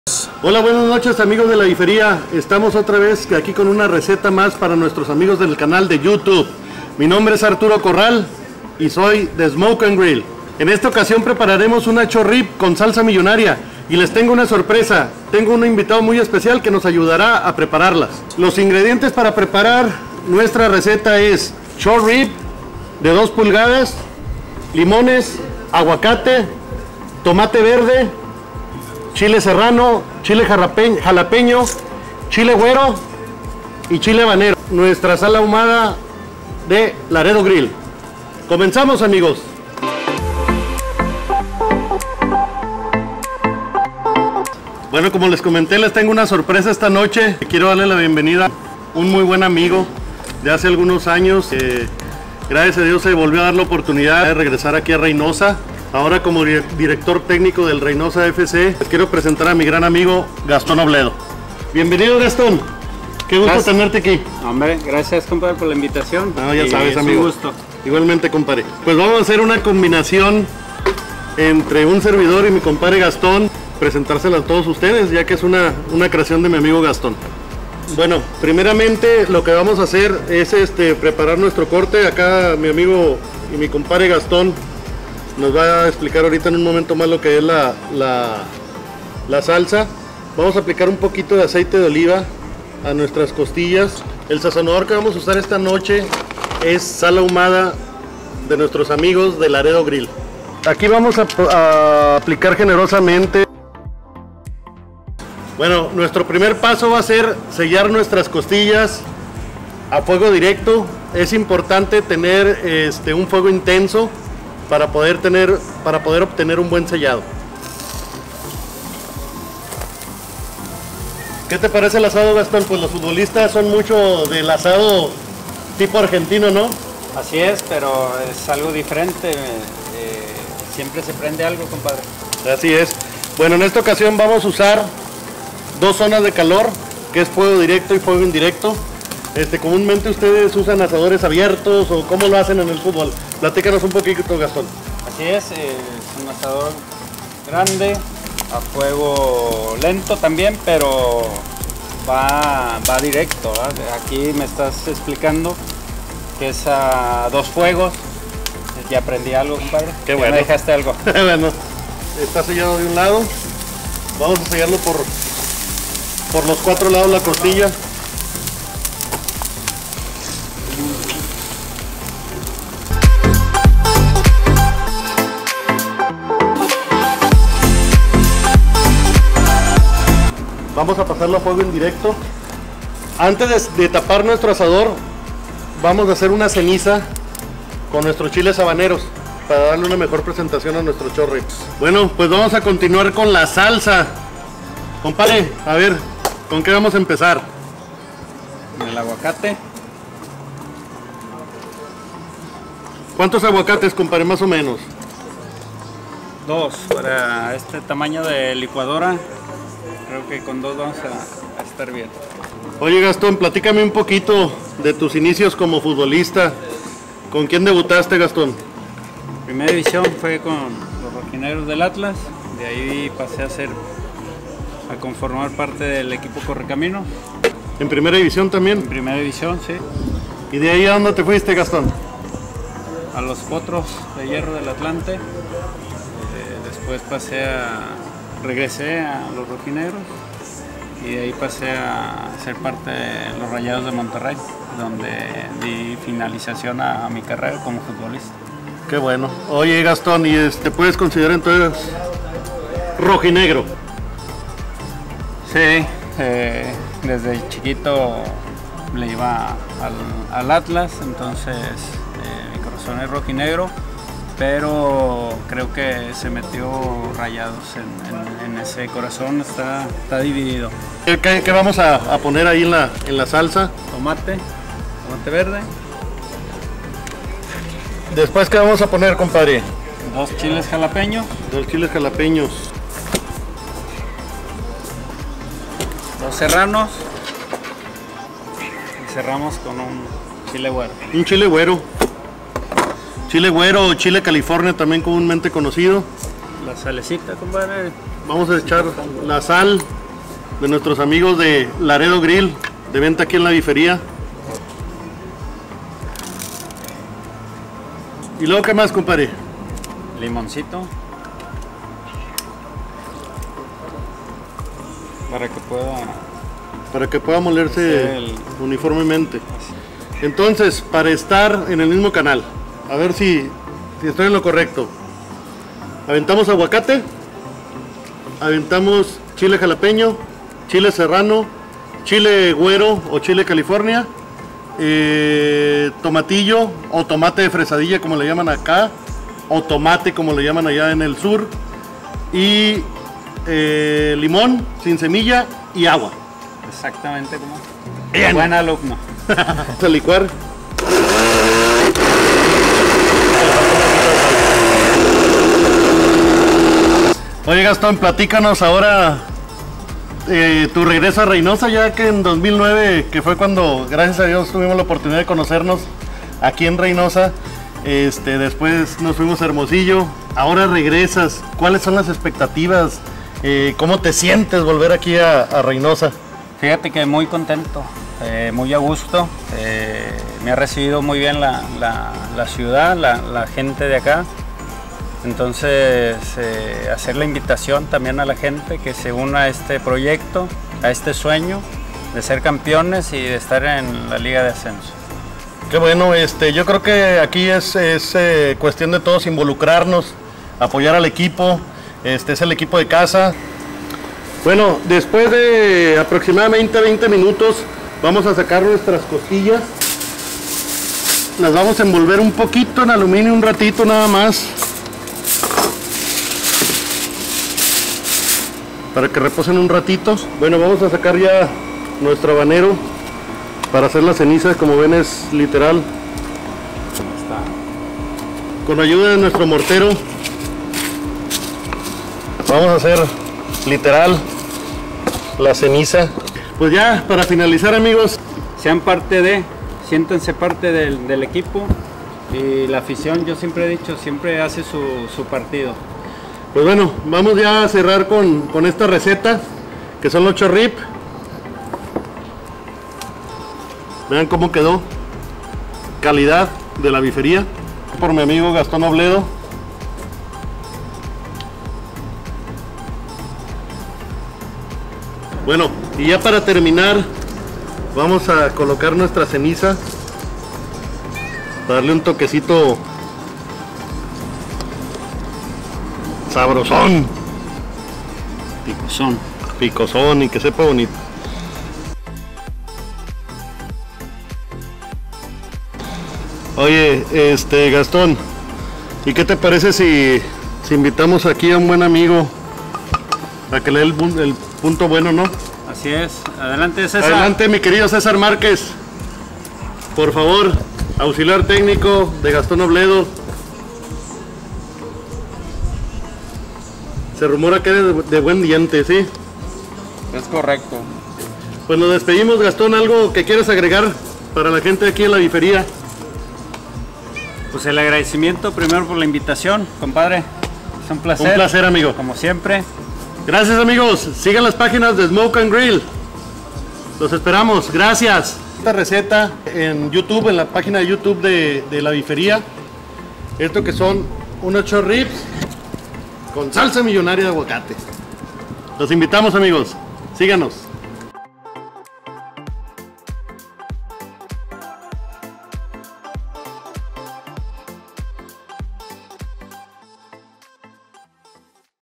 Short Rib con salsa millonaria de aguacate. Gastón Obledo y Smoke & Grill
En esta ocasión nuestro amigo Arturo Corral de Smoke & Grill armó la carnita y tuvimos un invitado especial del ambiente futbolero: Gastón Obledo; director técnico de Atlético Reynosa.

La receta en cuestión fueron unas jugosas short ribs o costillas cargadas acompañadas de una salsa de aguacate y diferentes chiles. Vean lo jugosas que salieron y juzguen ustedes mismos!

Que disfruten de esta carne asada !